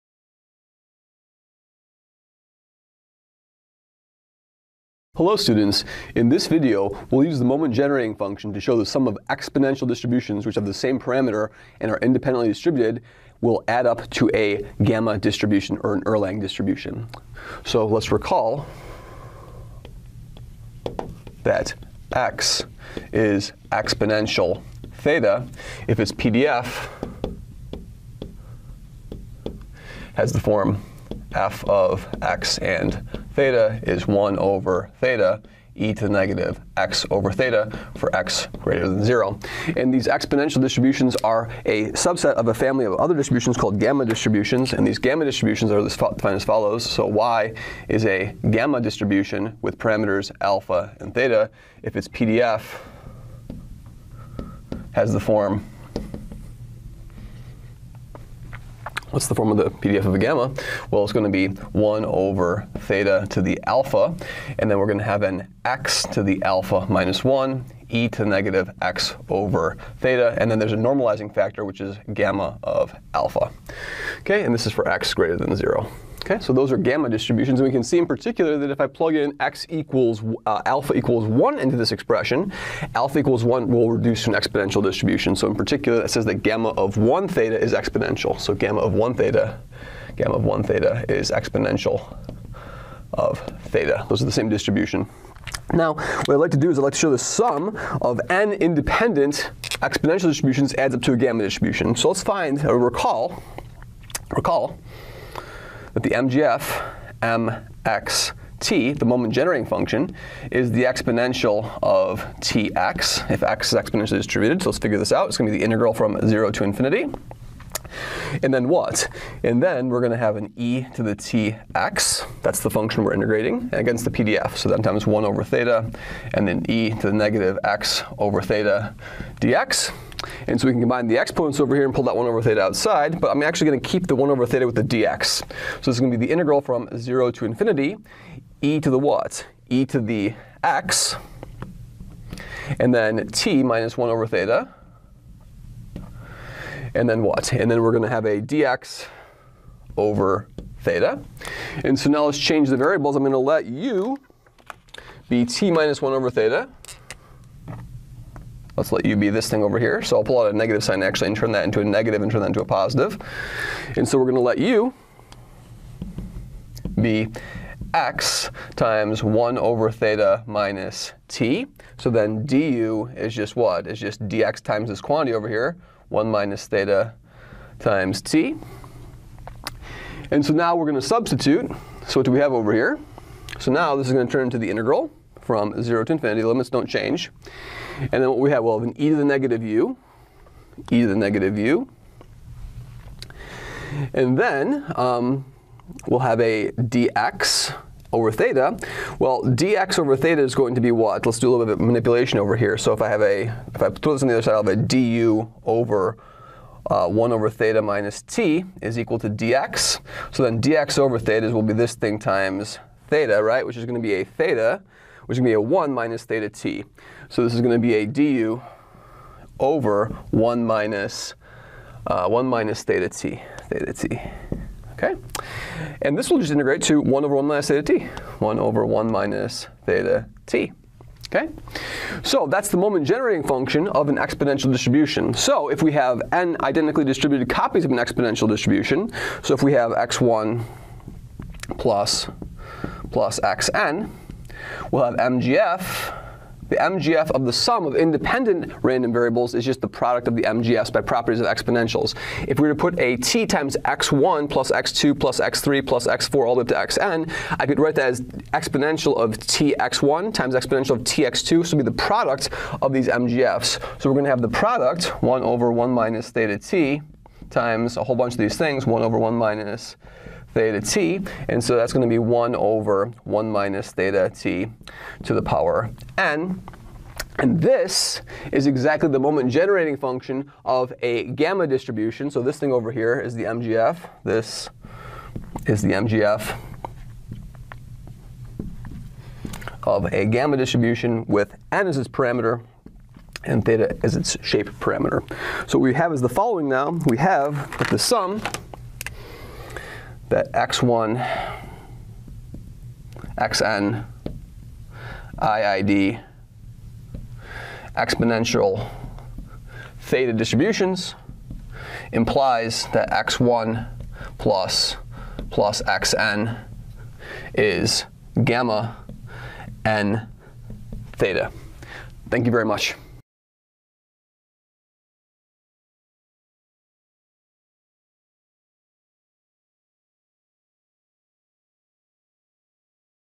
The Sum of Exponential Random Variables with Moment Generating Functions
We use the moment generating function of an exponential random variable to prove that the sum of independent exponential random variables follows a Gamma or Erlang distribution